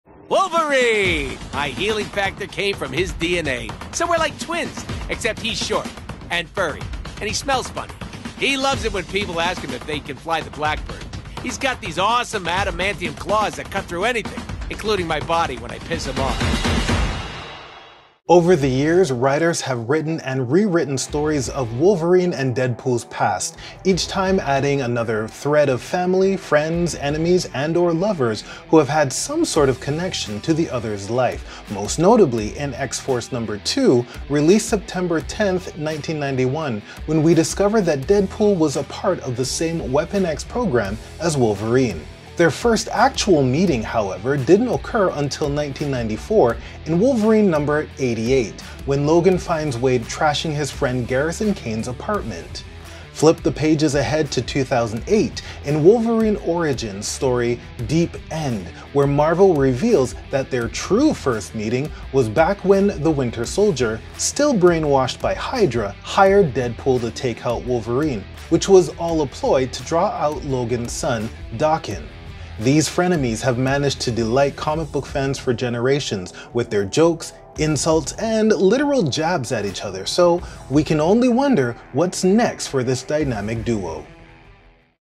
1st Meeting of Wolverine and Deadpool?
Did you know that Wolverine and Deadpool were connected even before they met each other? And their first meetings never go very well.